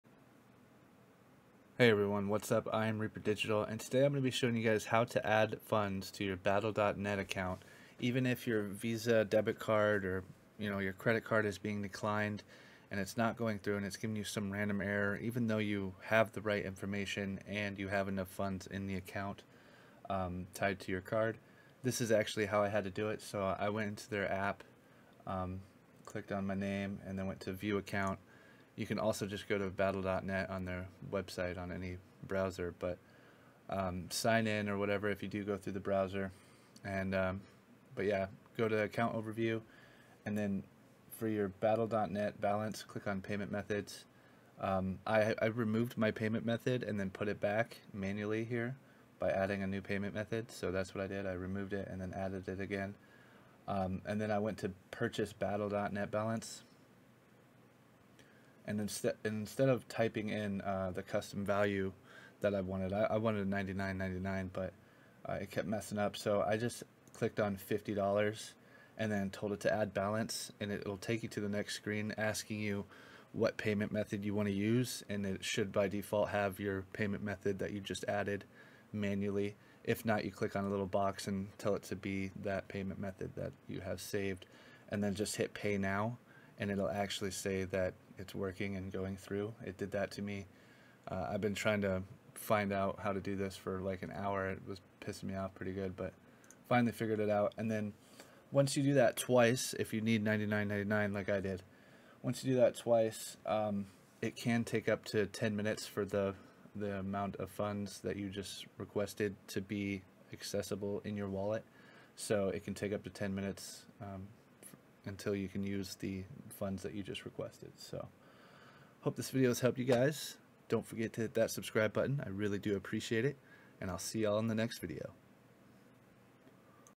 How to Fix Battle.net Add Funds Error 2022
In this video i show you all how to add funds to your Battle.net Wallet when it gives you an error despite the fact you know all the info is correct an that their are funds in your account.

ReaperDigital.